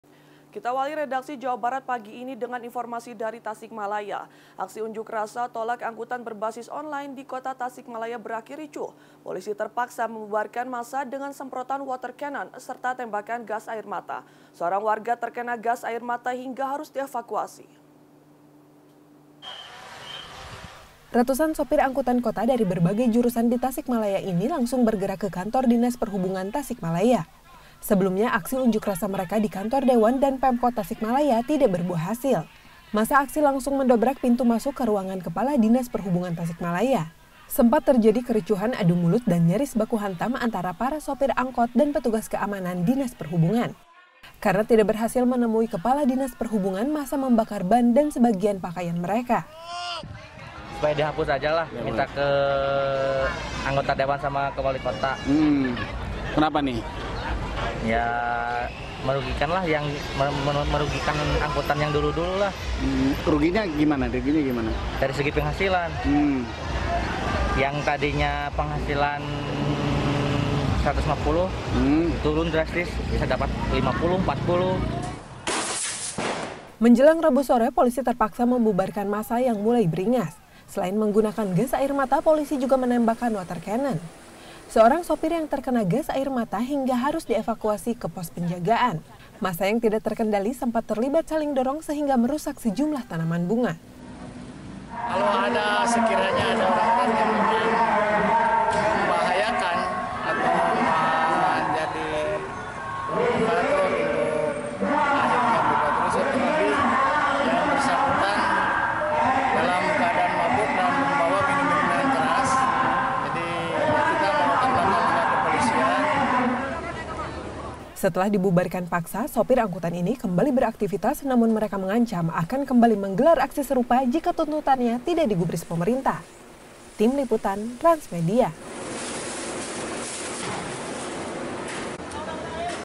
Polisi Membubarkan Sopir Angkot yang Demo, Menggunakan Gas Air Mata dan Water Canon.
Karena tuntutan menutup usaha angkutan berbasis online tidak juga dikabulkan, ratusan sopir angkot mendatangi kantor Dinas Pehubungan Tasikmalaya. Aksi sempat ricuh ketika masa mendobrak pintu ruang Kadis. Karena kesal tidak berhasil bertamu kadis, massa membakar ban di depan Kantor Dishub.

Follow IG kami di:
transmediajabar